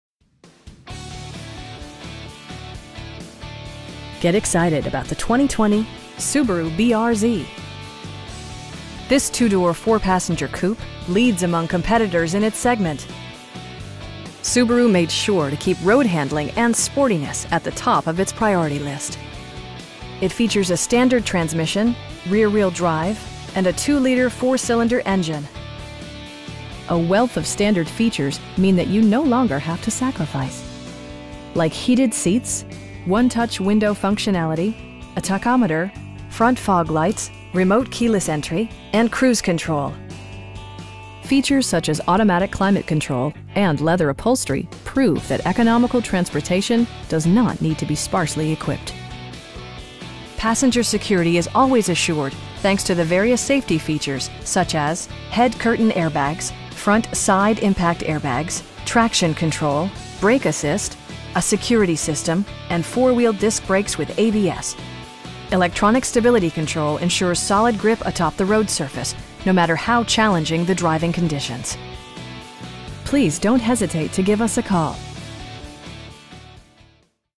2020 Subaru BRZ Limited in Salt Lake City, UT 84115
Get excited about the 2020 Subaru BRZ. This 2 door, 4 passenger coupe leads among competitors in its segment. Subaru made sure to keep road-handling and sportiness at the top of it's priority list. It features a standard transmission, rear-wheel drive, and a 2 liter 4 cylinder engine. A wealth of standard features means that you no longer have to sacrifice. Like heated seats, 1-touch window functionality, a tachometer, front fog lights, tilt and telescoping steering wheel, power windows, remote keyless entry, and cruise control. Features such as automatic climate control and leather upholstery prove that economical transportation does not need to be sparsely equipped. Passenger security is always assured thanks to various safety features, such as: head curtain airbags, front side impact airbags, traction control, brake assist, a security system, and 4 wheel disc brakes with ABS. Electronic stability control ensures solid grip atop the road surface, no matter how challenging the driving conditions. We know that you have high expectations, and we enjoy the challenge of meeting and exceeding them! Please don't hesitate to give us a call.